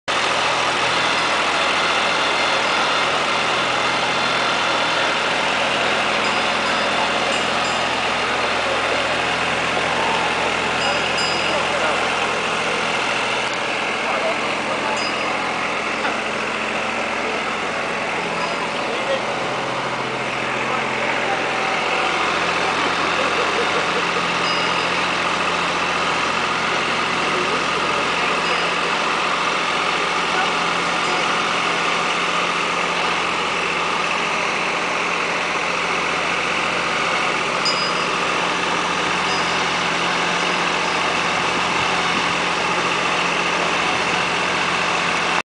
Spring Spectacular Hayride at Natick Community Organic Farm
A short clip of our very popular hayride. Thanks to Mike Umbrell for driving...